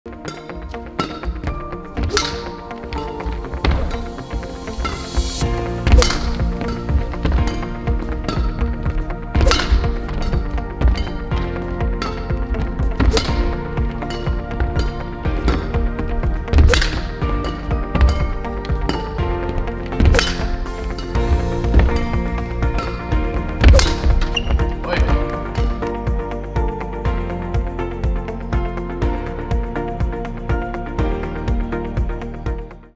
BACKHAND slice
BACKHAND slice. Enjoy these great backhand slices with a few technical tips. This is a great exercise to practice the small details in the slice 💯